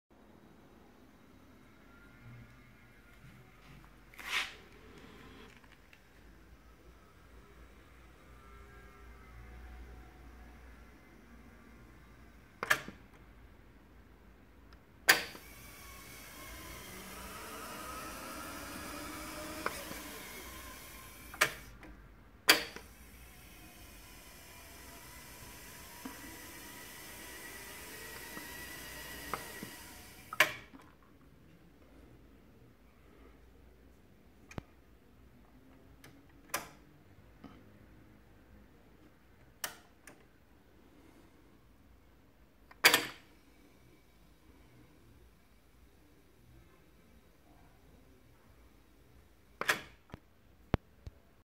New 5 digits counter for the Technics RS-1500/1700.
A couple of years ago I developed the Digital Real Time Counter for this R2R series.

At the time the only available display that fitted the narrow counter window in these decks was a  4 digits 0.2in.

Now a new display with 6 digits 0.2in. can be used for this application allowing a more standard representation in the form: H.MM.SS with no need to file trim the counter window width, while keeping the same accuracy.

Only drawback is that these displays can only be Blue or Red at the moment.